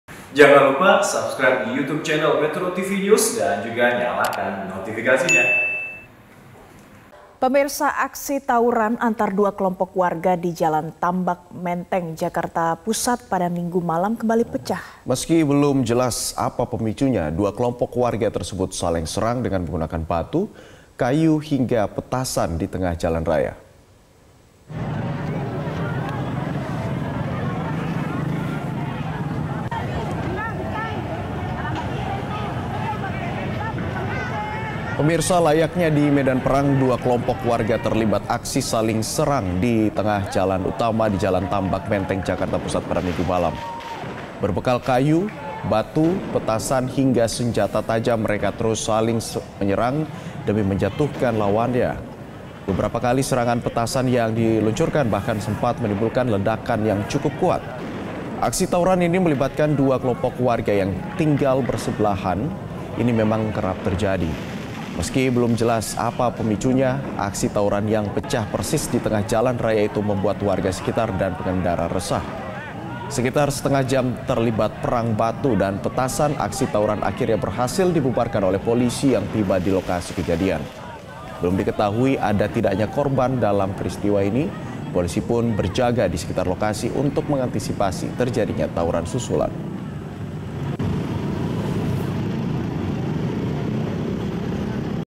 Aksi Saling Serang Antar Kelompok Warga di Menteng
Aksi tawuran antar dua kelompok warga di Jalan Tambak, Menteng, Jakarta Pusat, Minggu (18/9/2022) malam kembali terjadi.

Meski belum jelas pemicunya, dua kelompok warga tersebut saling serang menggunakan batu, kayu hingga petasan di tengah jalan raya. Layaknya di medan perang, dua kelompok warga terlibat aksi saling serang.

Berbekal batu, kayu, petasan hingga senjata tajam mereka terus saling serang demi menjatuhkan lawannya. Beberapa kali serangan petasan yang diluncurkan bahkan sempat menimbulkan ledakan yang cukup kuat.

Sekitar setengah jam terlibat perang batu dan petasan, aksi tawuran akhirnya berhasil dibubarkan polisi yang tiba di lokasi kejadian. Belum diketahui ada tidaknya korban dalam peristiwa ini. Polisi pun berjaga di sekitar lokasi untuk mengantisipasi terjadinya tawuran susulan.